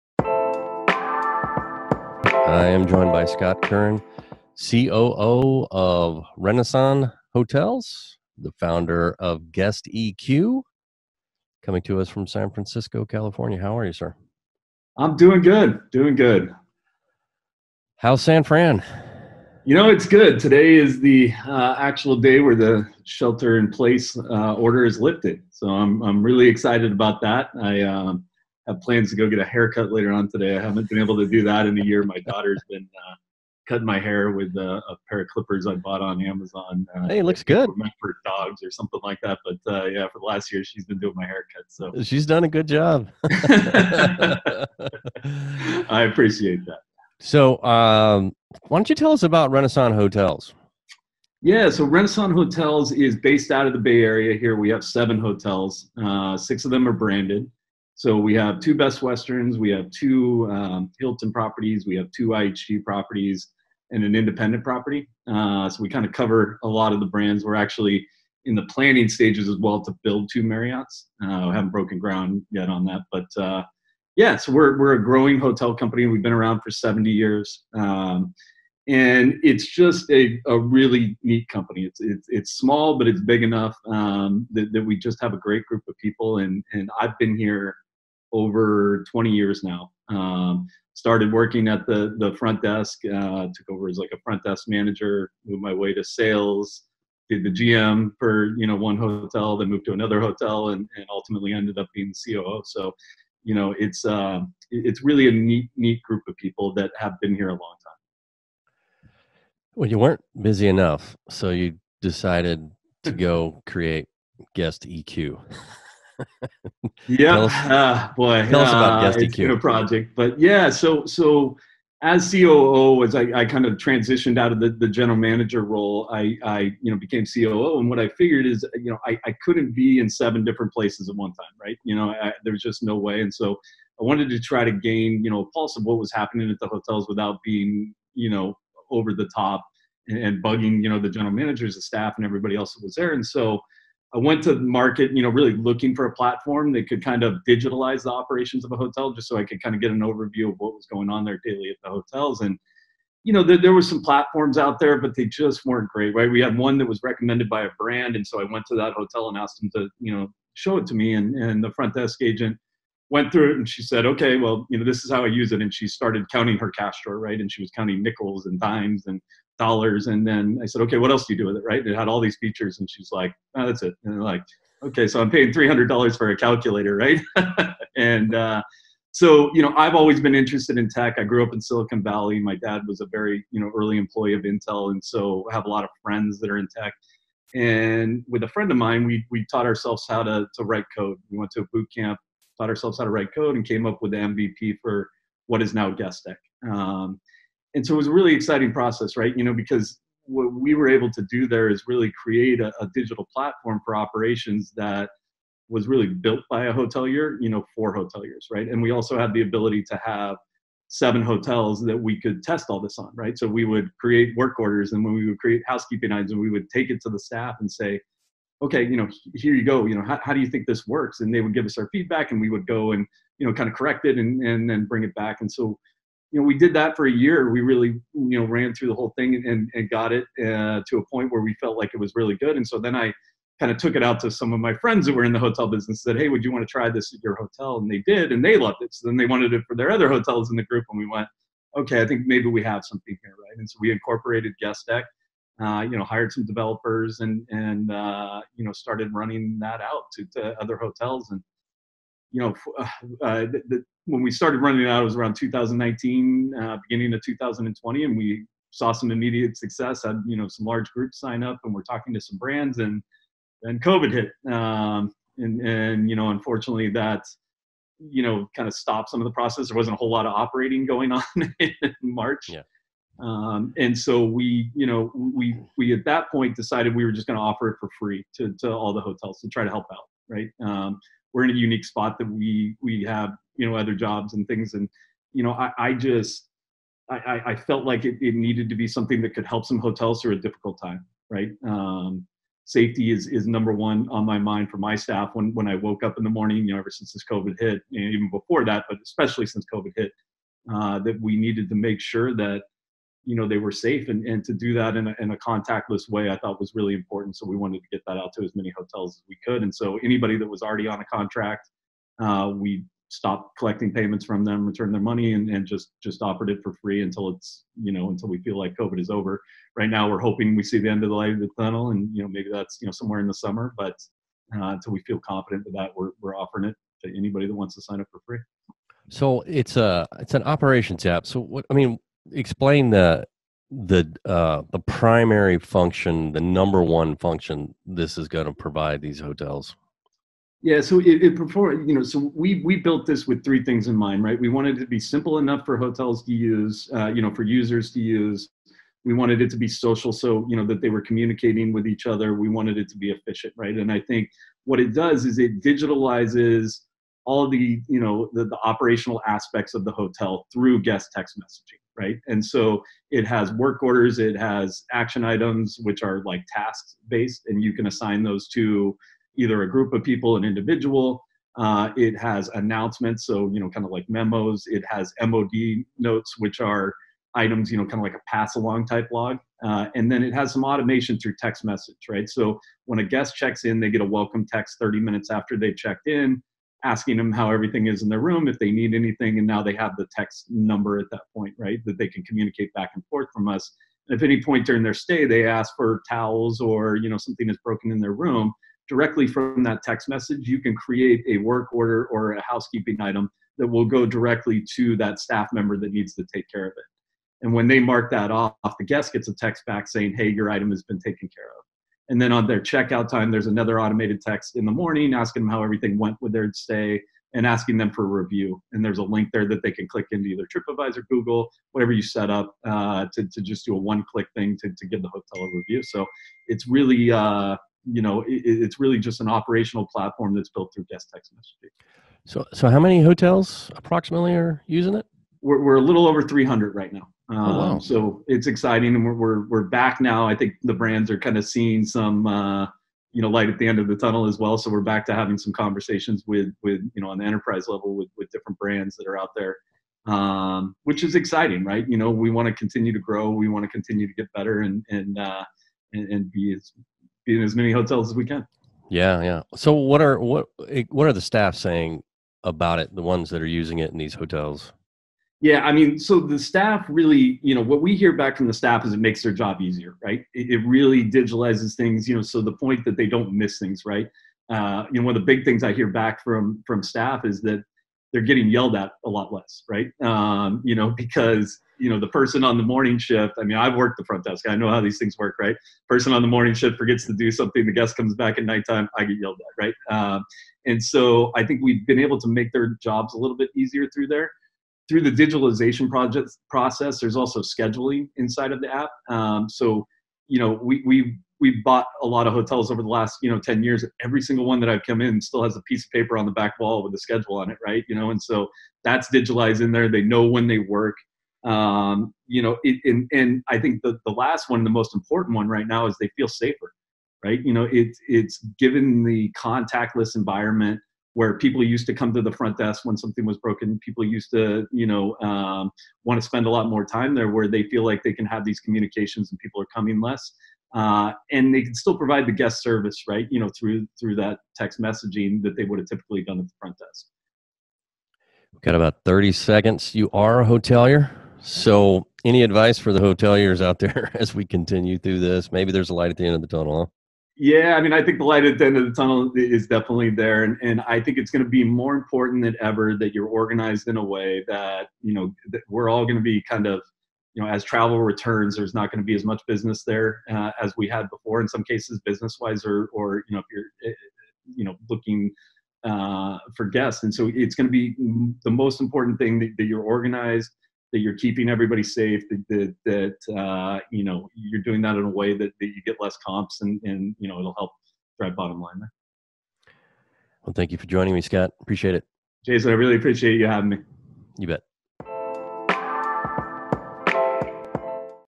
SHR Soundbites: Better hotel operations
Scott Curran, COO of Reneson Hotels and Founder of GuestEQ, joins Jason from San Francisco, California.

Scott talks about his 20 years at Reneson, teaching himself to code, and the creation of his Operations and Guest Text platform, GuestEQ.